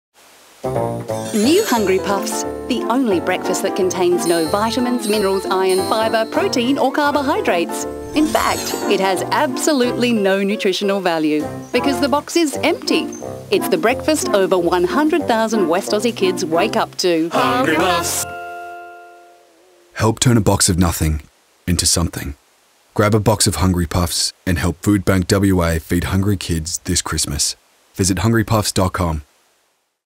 Hungry Puffs 30 sec TVC
Hungry Puffs - is the empty box of cereal that is helping feed hungry kids.